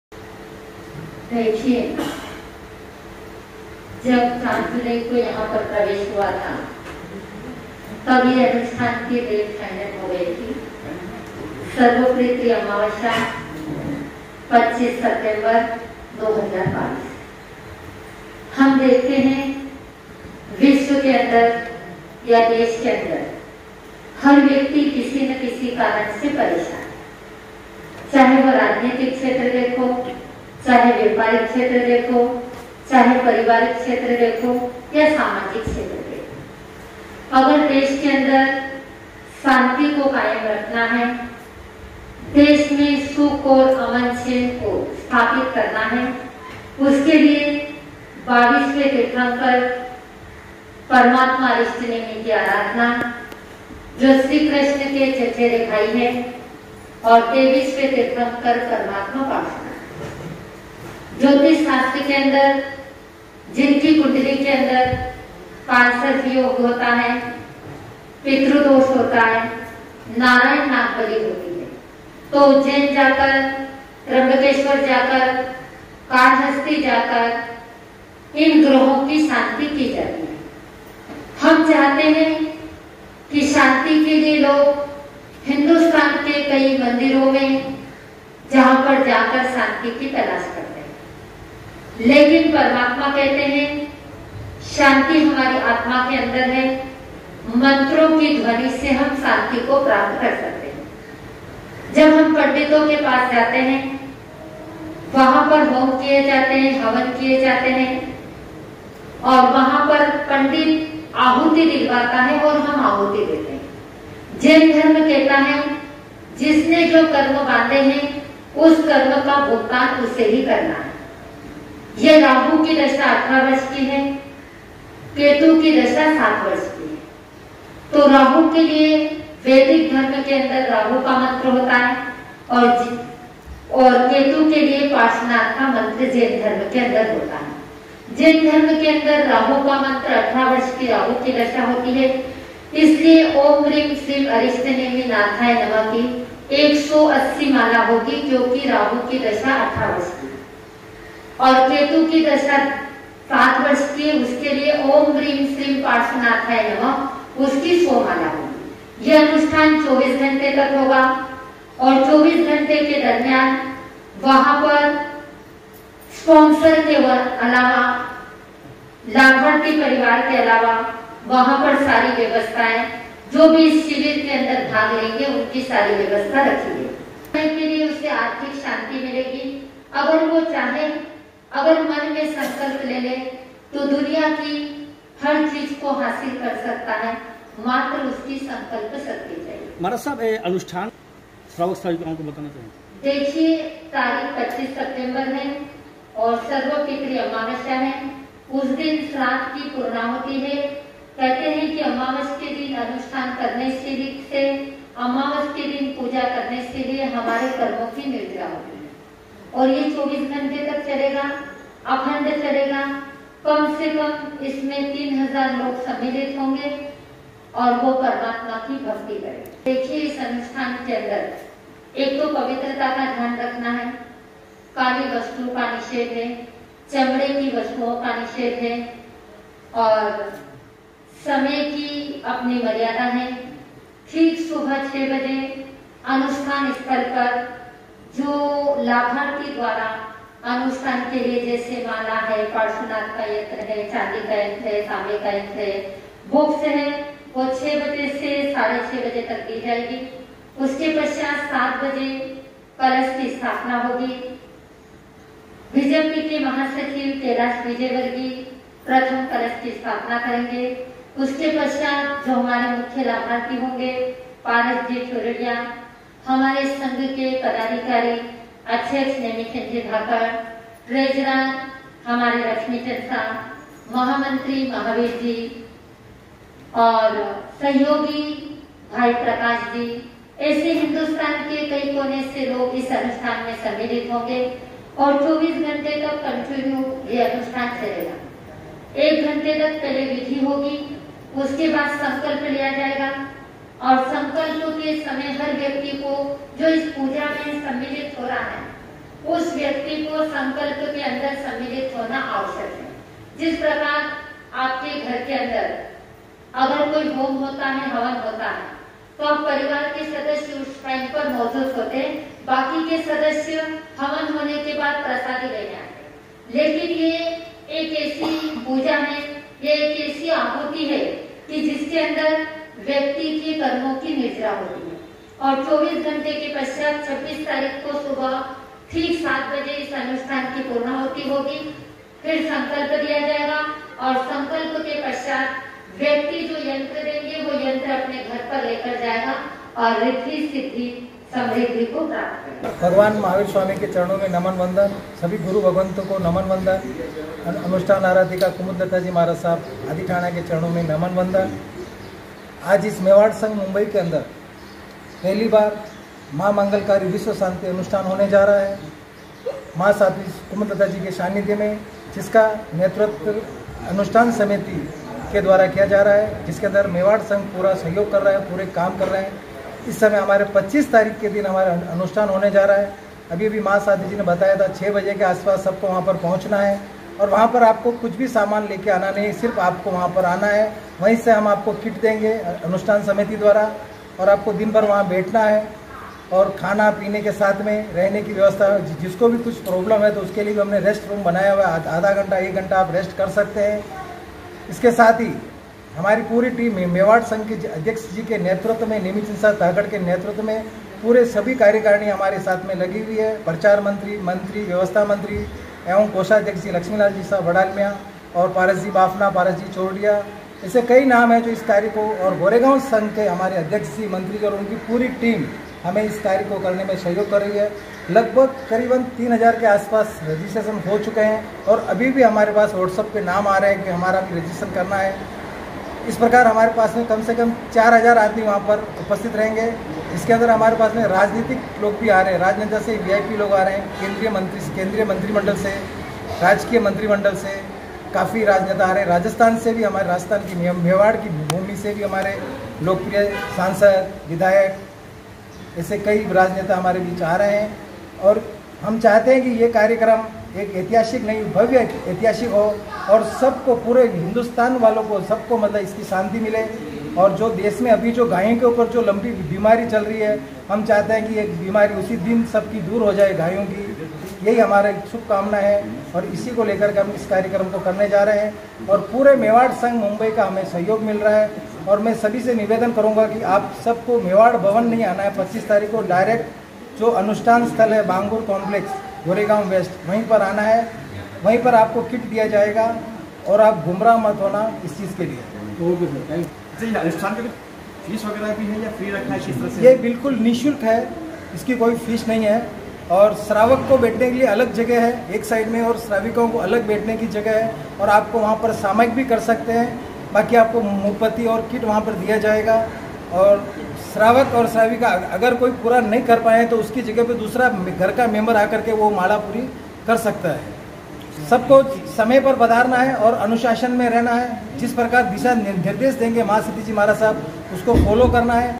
साध्वी कुमुद लता के सानिध्य में लोग करेंगे साधना, मेवाड़ संघ द्वारा होगा अनुष्ठान
गोरेगांव में महा मंगलकारी अनुष्ठान की जोरदार तैयारियाँ

साध्वी कुमुद लता के सानिध्य में हजारों लोग करेंगे साधना, मेवाड़ संघ द्वारा बांगुर स्पोर्ट्स कॉम्प्लेक्स में हो रहा है भव्य अनुष्ठान

मुंबई। गोरेगांव पश्चिम में 25 सितंबर को महासती कुमुदलता महाराज के सानिध्य में आयोजित होने जा रहे महामंगलकारी अनुष्ठान की जोरदार तैयारियां चल रही है। इस बात की जानकारी देते हुए साध्वी कुमुदलता जी महाराज ने मुंबई की मीडिया को संबोधित किया और कहा कि विश्व कल्याण और गोमाता को लम्पी वायरस से बचाने के लिए यह अनुष्ठान आयोजित हो रहा है।
मेवाड़ भवन में साध्वीश्री कुमुद लता जी महाराज के सांनिध्य में हुई प्रेस काफ्रेंस में मेवाड संघ के महामंत्री महावीर तातेड़ ने बताया कि कार्यक्रम की तैयारियों को लेकर अनुष्ठान समिति अध्यक्ष नेमीचंद धाकड़ के नेतृत्व में संघ के पदाधिकारियों ने आयोजन स्थल जाकर वहां चल रही तैयारियों का जायजा लिया। कार्यक्रम स्थल पर बन रहे विशाल डोम में बैठक व्यवस्था, भोजन व्यवस्था, पार्किंग व्यवस्था और अतिथि सत्कार व्यवस्था आदि का निरीक्षण किया गया।
तातेड़ ने बताया कि विभिन्न राज्यों से पदाधिकारियों के मुंबई पहुंचने का सिलसिला शुरू हो गया है। अनुष्ठान को सफल बनाने के लिए मेवाड़ भवन में बैठकें आयोजित की जा रही हैं। कार्यक्रम में मेवाड़ संघ की अहम भूमिका हैं और विश्व शांति के लिए किया जा रहा यह कार्यक्रम स्वर्णिम इतिहास रचेगा। प्रेस काँफ्रेंस में संघ के कोषाध्यक्ष लक्ष्मी लाल वडालमिया, व्यवस्था मंत्री प्रकाश नाहर और पारसमल बाफना भी मौजूद रहे।